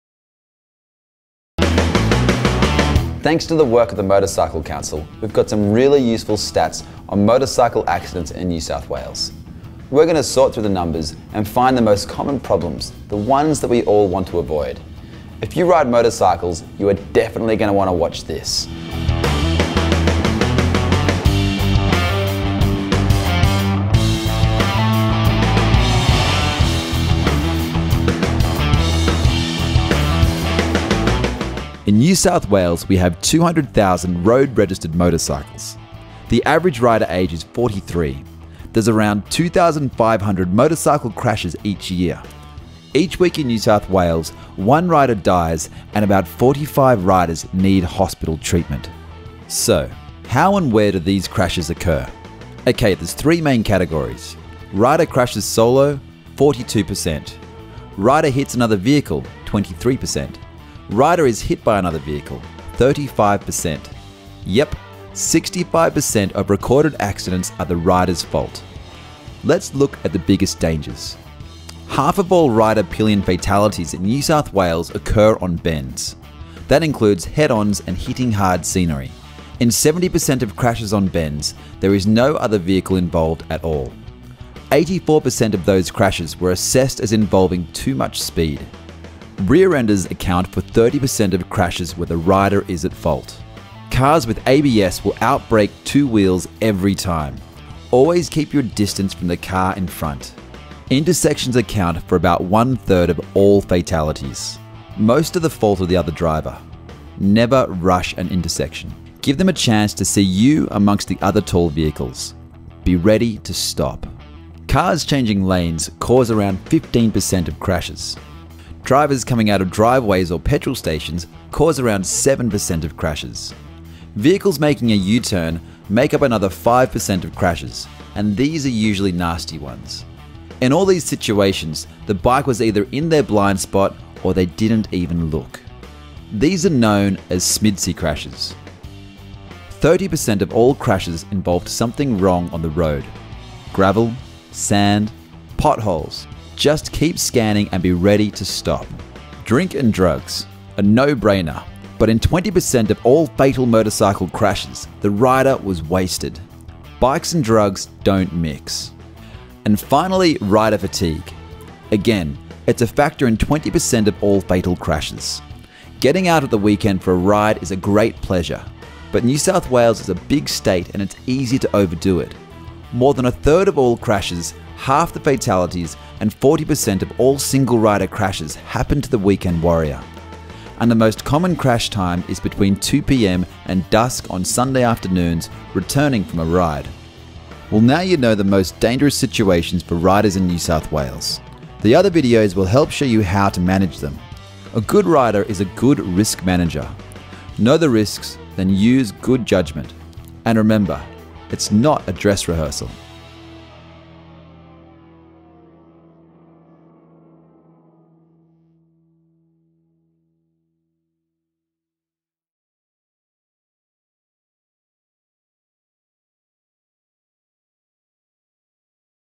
RIDER_RISK_CLASSIC_CRASH_SCENARIOS
Here are some scenarios played out - watch this to help you avoid making the same mistakes.